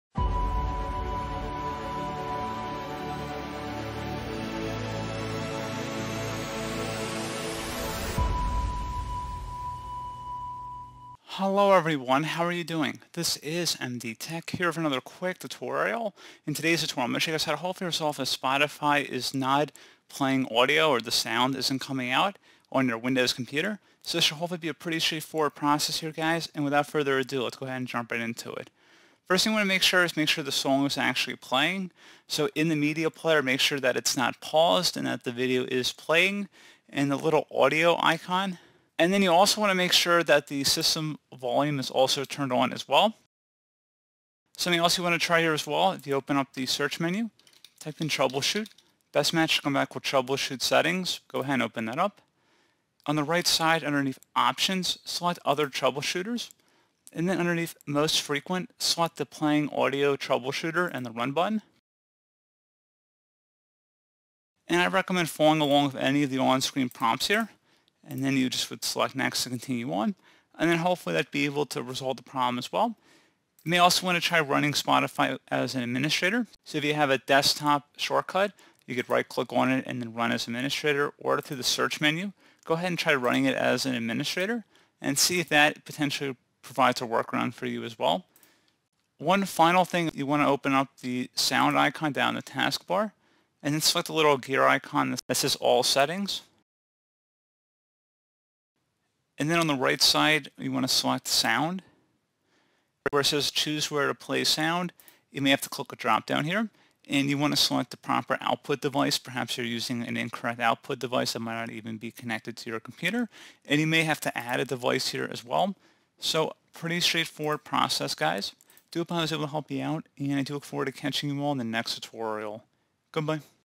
Fix No Sound In Spotify On Windows 11 PC [Tutorial]
Spotify is a leading music streaming platform.  However, a lot of users are finding it hard to use the app on a Windows platform. According to them, when they play a song, they hear no sound in Spotify. If you are facing a similar issue, check this tutorial for solutions.

Spotify is a music-playing application that anybody may use to listen to music.

Many Windows users have lately reported their dissatisfaction with the fact that when they played music or song in the Spotify app on a Windows device, they are unable to hear any sound from the device.